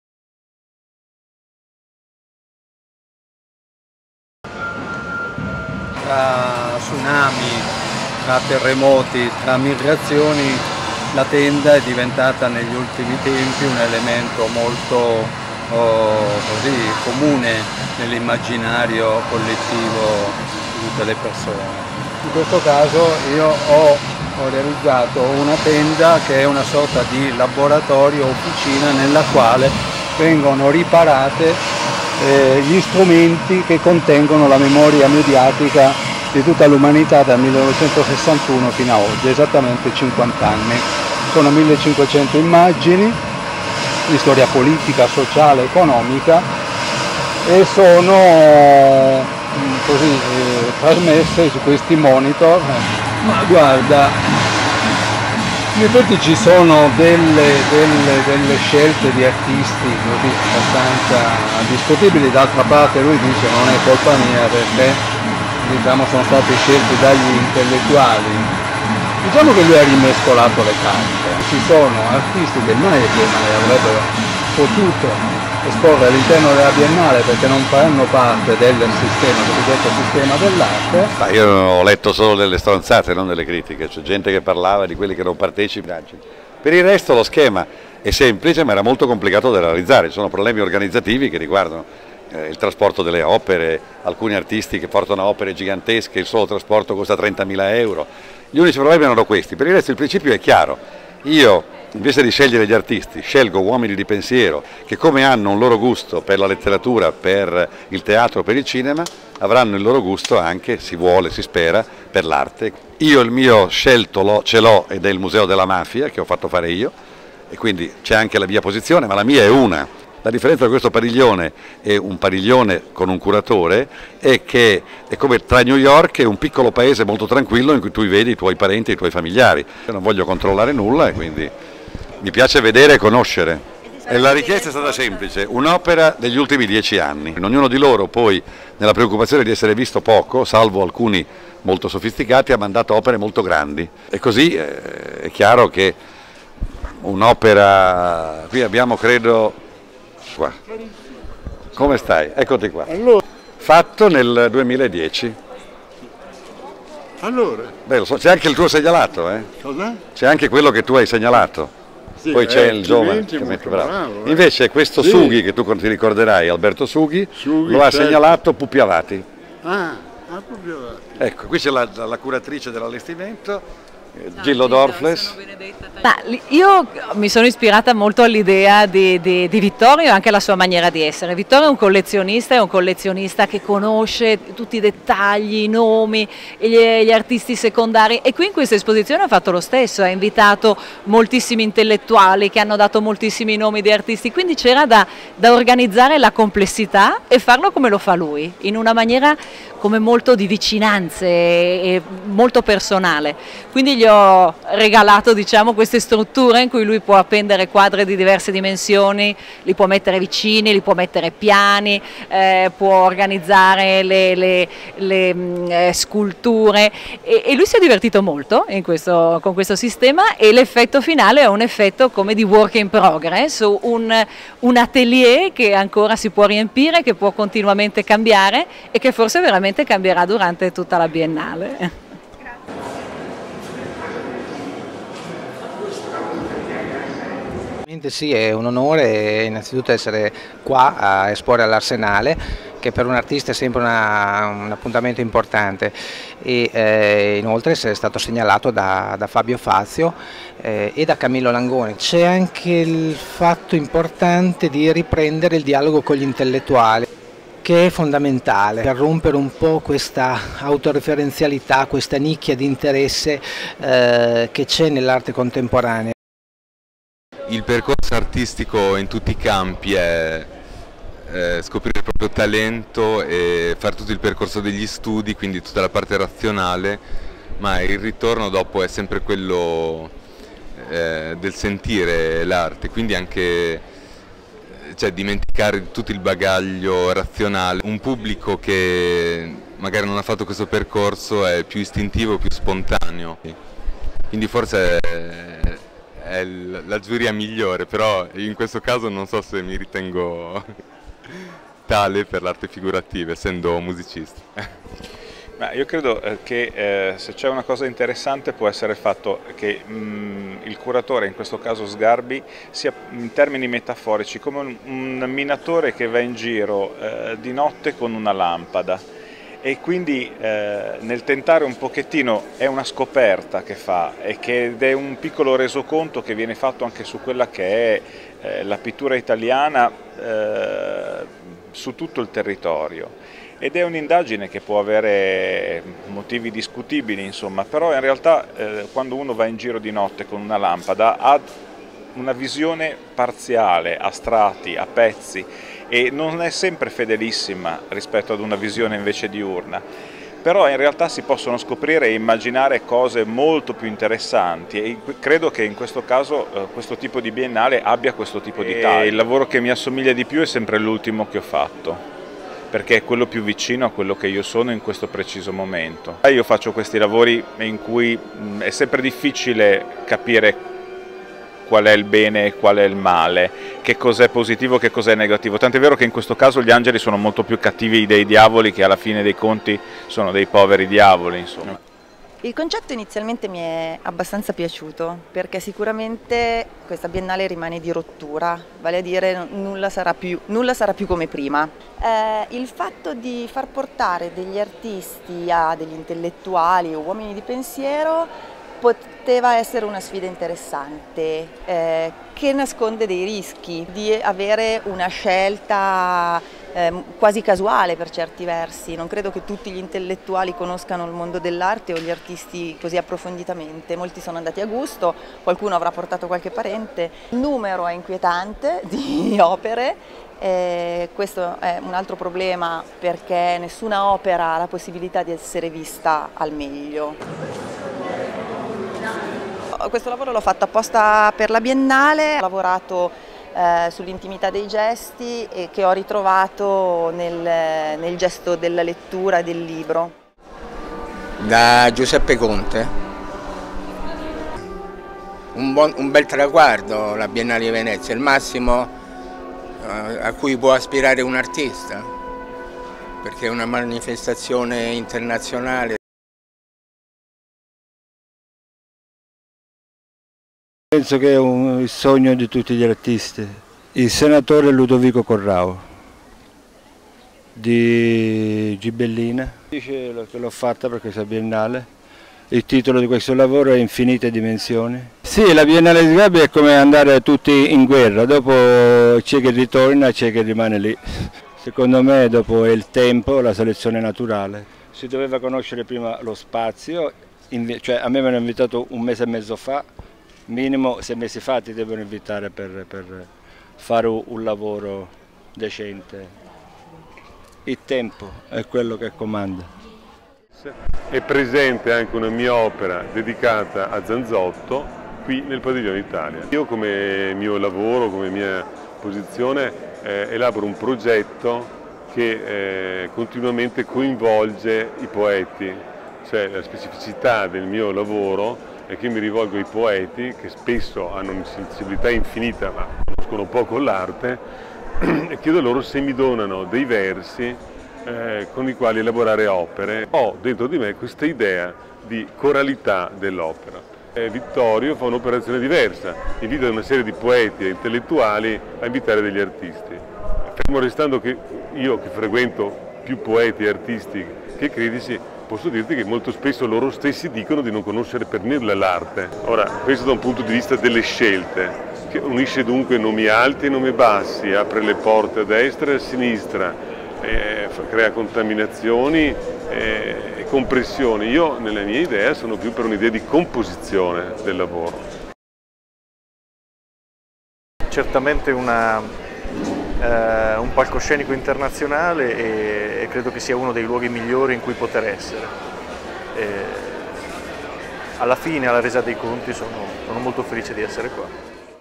Biennale 2011 Padiglione Italia parte prima
Incontri con artisti presenti al Padiglione Italia, Biennale 2011, impressioni, conversazioni, opere.